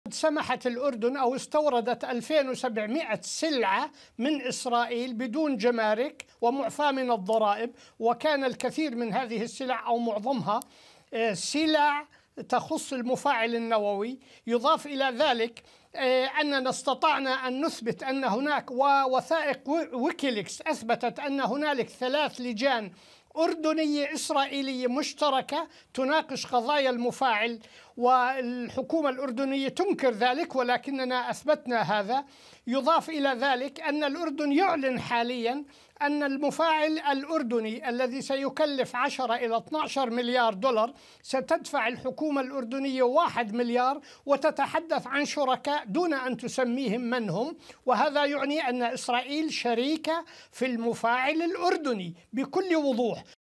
هل هناك شراكة نووية بين إسرائيل والأردن؟
الخبير البيئي الأردني سفيان التل يتهم بلاده بالتنسيق مع إسرائيل في المجال النووي رغم نكران الحكومة الأردنية لذلك.